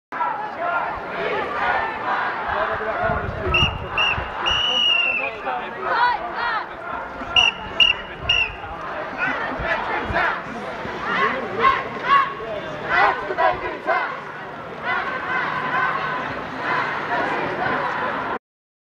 Quays News - Manchester Anti-Bedroom Protest March (16th March)
Footage of Anti-bedroom tax March in Manchester (16th March)